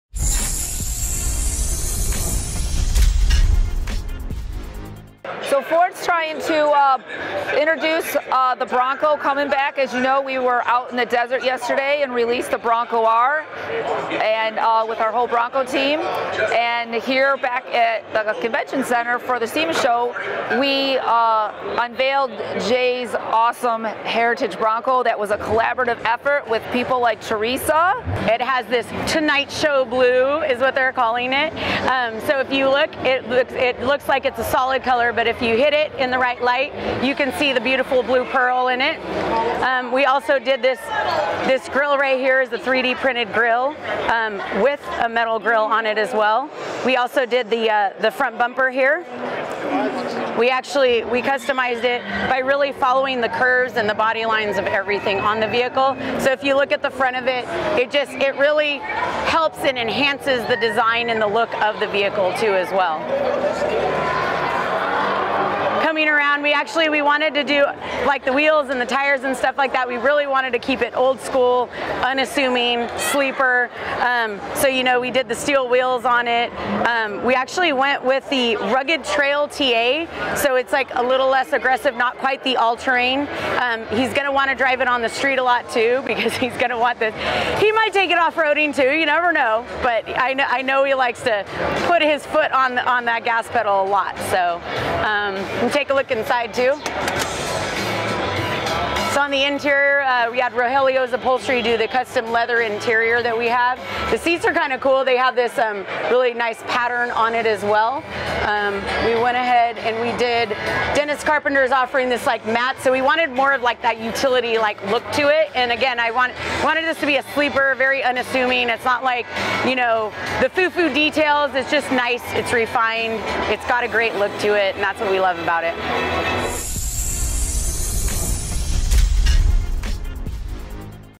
Jay Leno's Bronco Unveiled
On Day One of the 2019 SEMA Show, Jay Leno appeared in the Ford booth to unveil a very special Bronco.